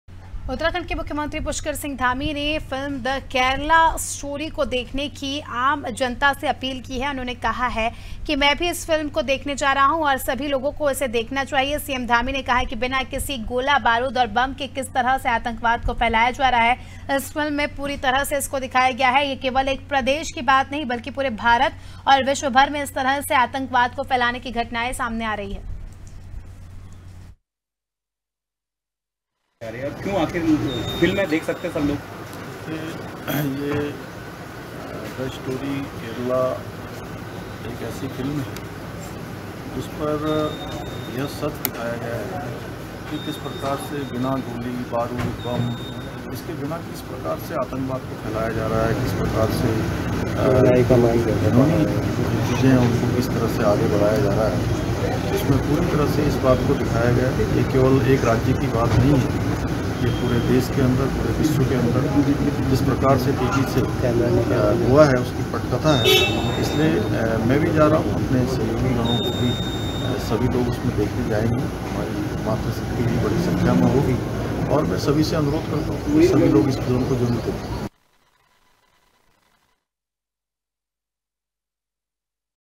Dehradun : "द केरल स्टोरी" पर बोले CM धामी, ‘बिना बारूद के फैलाया जा रहा आतंकवाद’ || Anaadi Tv
उत्तराखंड के मुख्यमंत्री पुष्कर सिंह धामी ने फिल्म "द केरला स्टोरी" को देखने की आम जनता से अपील की है...उन्होंने कहा है कि, मैं भी इस फिल्म को देखने जा रहा हूं...और सभी लोगों को इसे देखना चाहिए...सीएम धामी ने कहा कि, बिना किसी गोला, बारूद और बम के किस तरह से आतंकवाद को फैलाया जा रहा है...इस फिल्म में पूरी तरह से इसको दिखाया गया है...यह केवल एक प्रदेश की बात नहीं है बल्कि पूरे भारत और विश्व भर में इस तरह से आतंकवाद को फैलाने की घटनाएं सामने आ रही हैं...

COMING SOON
 DEN, AIRTEL